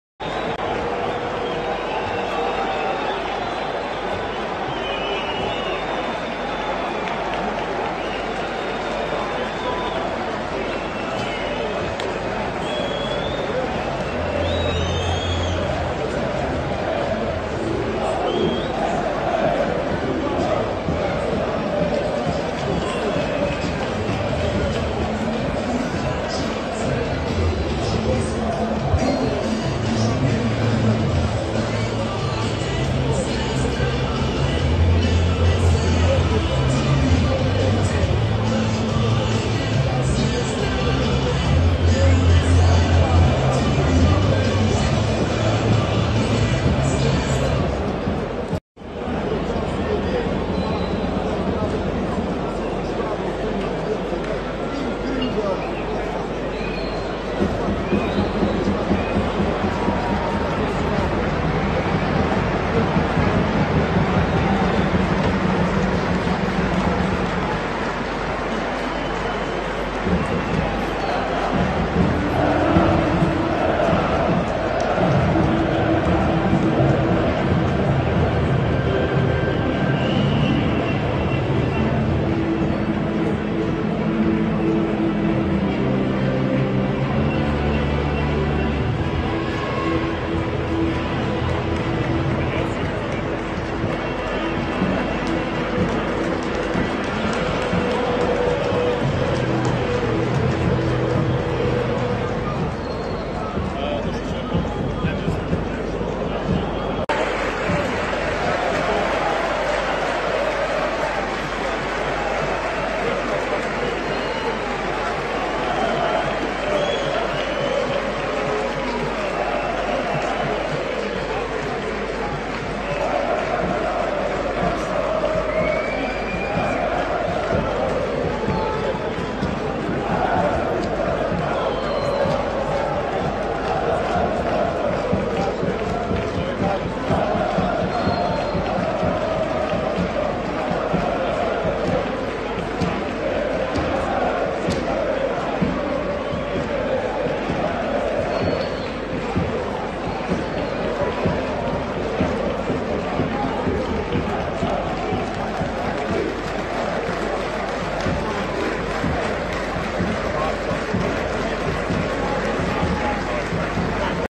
Bruno Lage festeja exuberantemente com a vitória (1-2) do Benfica em casa do Estrela Vermelha.
Bruno Lage ficou completamente eufórico com a vitória (1-2) do Benfica em casa do Estrela Vermelha. Mal soou o apito final, o técnico foi esbracejando enquanto se dirigia para junto dos adeptos encarnados, com quem celebrou efusivamente no triunfo no arranque desta Liga dos Campeões.